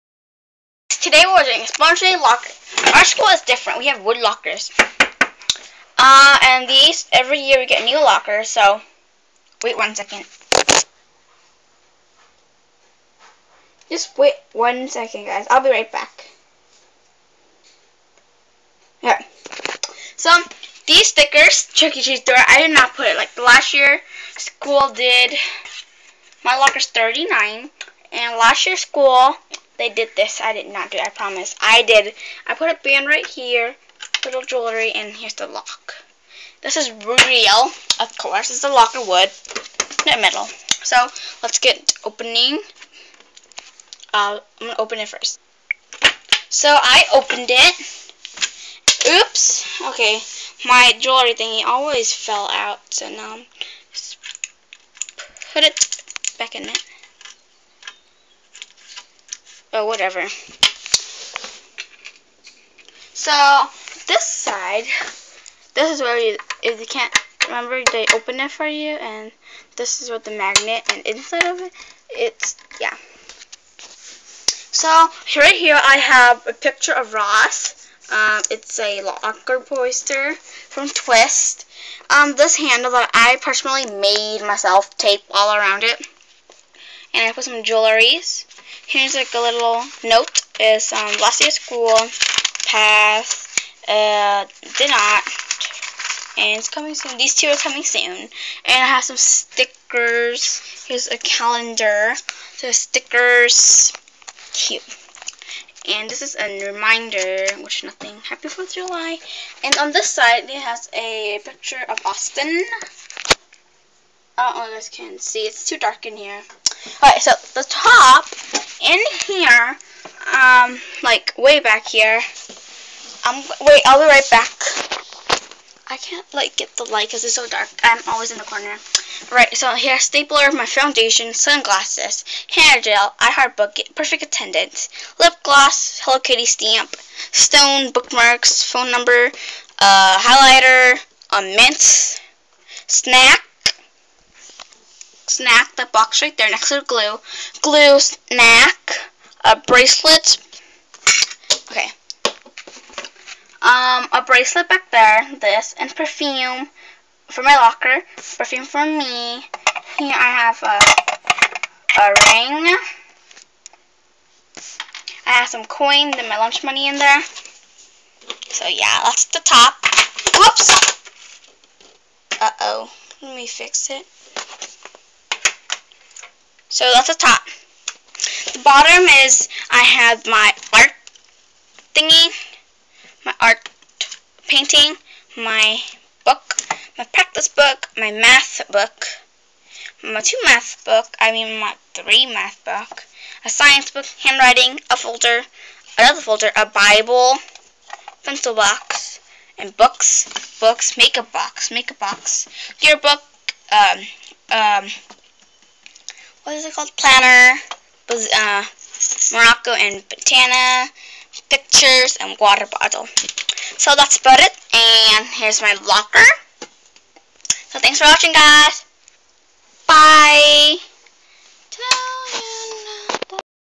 Locker tour
Lol this is my home locker that i made.